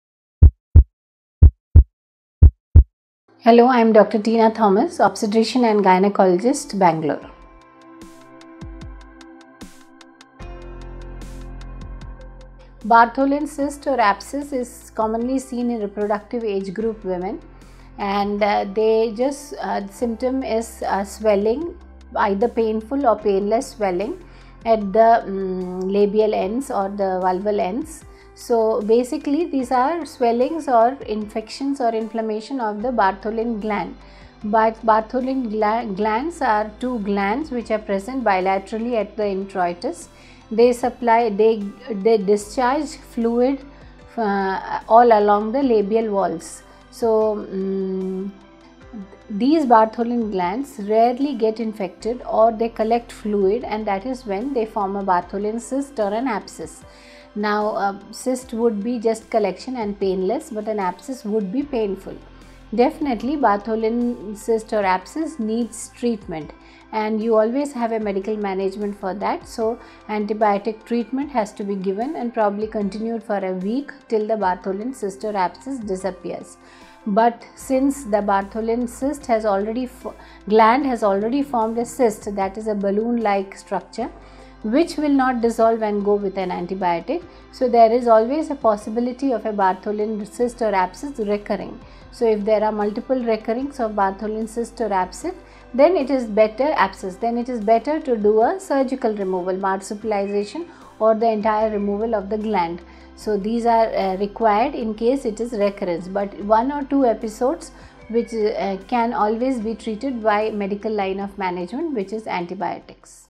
Can one manage cyst in pubic region by medications? - Dr. Teena S Thomas
Bartholin’s cyst or abscess is usually seen in reproductive age group in women and the symptom is painful or painless swelling at the labial ends or the vulval ends. So basically these swellings or infections or inflammations of the Bartholins glands. Bartholins glands aretwo glands which ar epresent in the introitus. They discharge fluid all along the labial walls. These Bartholin’s glands rarely they get infected and they collect fluid. That is when they form a Bartholin’s cyst or abscess. Cyst would be just a collection and it would be painless and abscess would be painful. Bartholin’s cyst or abscess needs treatment. So you have a medical management for that and antibiotic has to be given for that, continued for a week till the Bartholins cyst or abscess disappears. But the Bartholin’s glands has formed a cyst, which will not get cured with an antibiotic. So there is a probability of recurrence of Bartholin’s cyst or abscess. If there are multiple recurrence of Bartholin’s cyst or abscess, then it is better to do a surgical removal. Marsupialisation or the entire removal of the gland. This is required once it is recurrent. One or two episodes can always be treated by medical line of treatment that is antibiotics.